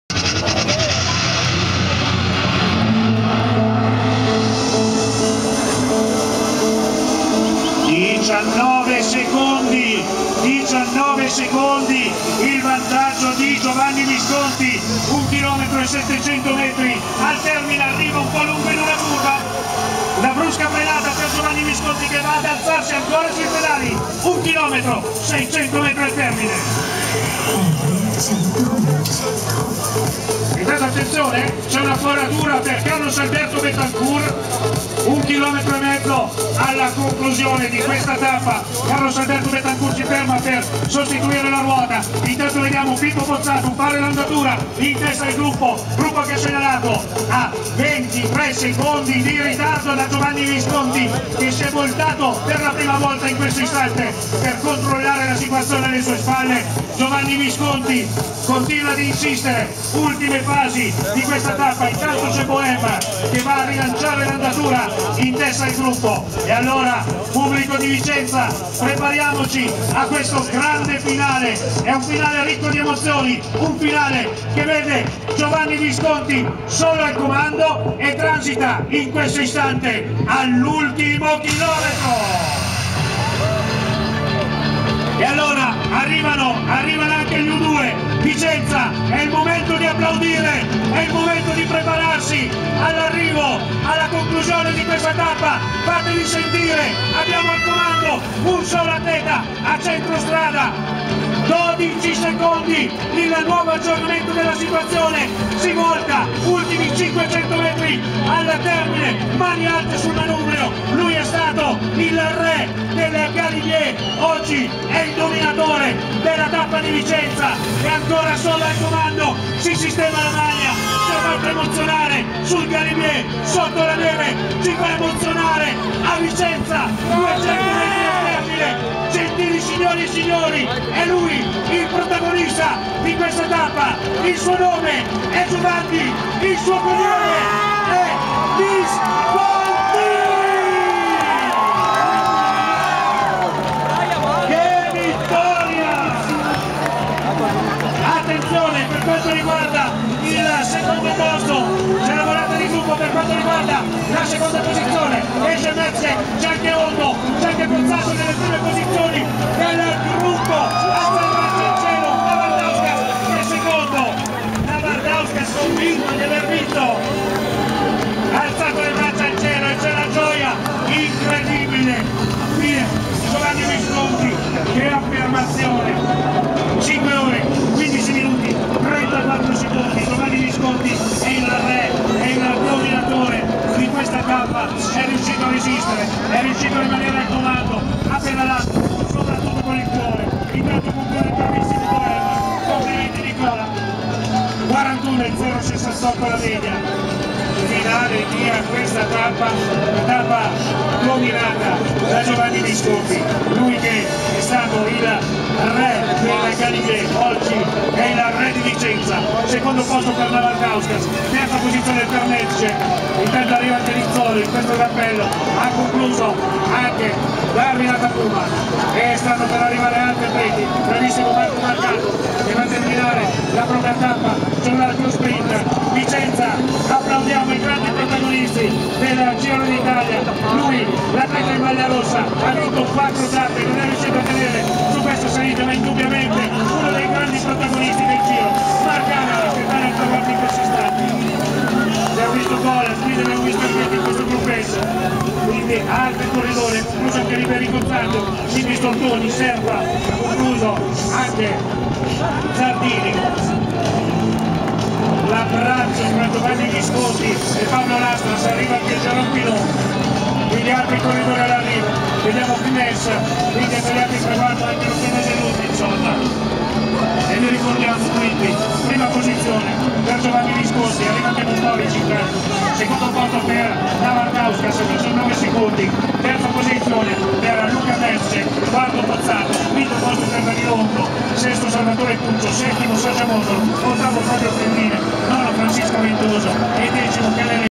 Giro d'Italia 2013, 17^ tappa: la vittoria di Giovanni Visconti a Vicenza
VICENZA, 22 MAGGIO 2013 - Dopo l'impresa e il trionfo sul Galibier, in Francia, Giovanni Visconti dimostra di essere tornato e va a vincere la sua seconda tappa di questo Giro d'Italia 2013. Arriva in solitaria sul traguardo di Vicenza, con le braccia al cielo, per un'altra vittoria liberatoria. Solo quarto il vicentino Pozzato, mentre Nibali conserva la maglia rosa. Tanta gente ai bordi delle strade, una meravigliosa giornata di sport nella calda e accogliente città vicentina.